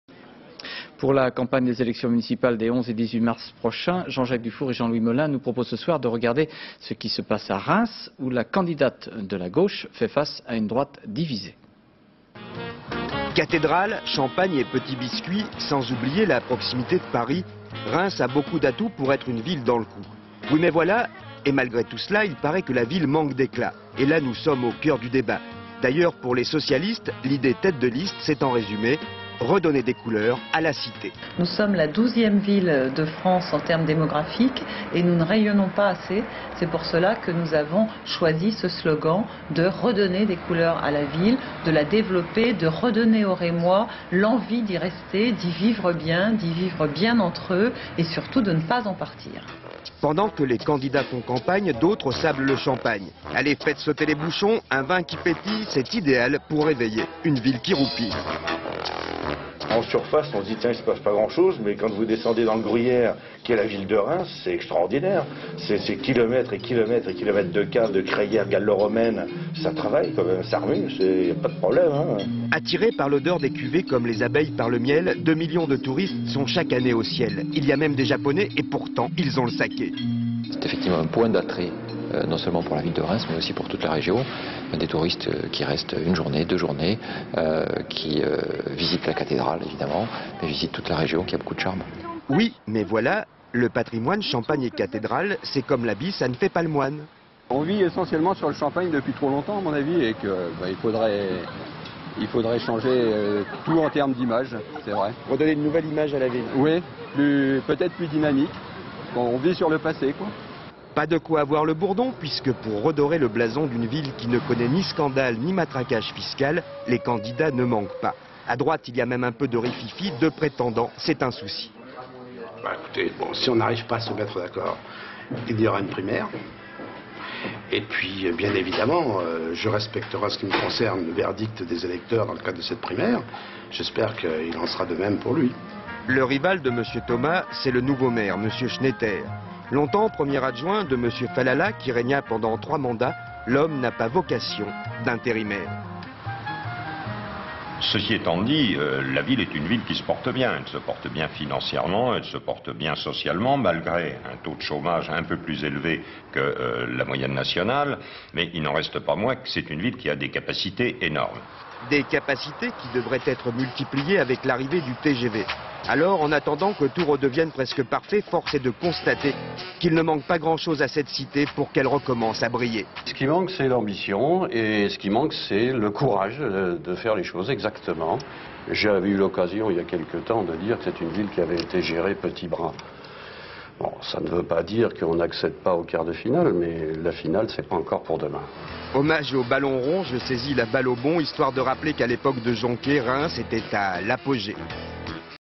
Reims : élections municipales
5 fevrier 2001
Reportage consacré à la campagne menée à Reims en vue des élections municipales des 11 et 18 mars prochains. La candidate de la gauche, Adeline HAZAN veut redonner des couleurs à la ville qui selon elle, ne valorise pas suffisamment ses atouts. Commentaire sur images de la campagne, de la ville, de caves de champagne, en alternance avec les interviews d'Adeline HAZAN, gauche plurielle, Jean-Claude THOMAS, candidat RPR, Jean-Louis SCHNEITER, maire sortant, candidat divers droite et de plusieurs chefs d'entreprises de la région. politique; archive television; archive tv; ina; inna; Institut National de l'Audiovisuel; french tv Images d'archive INA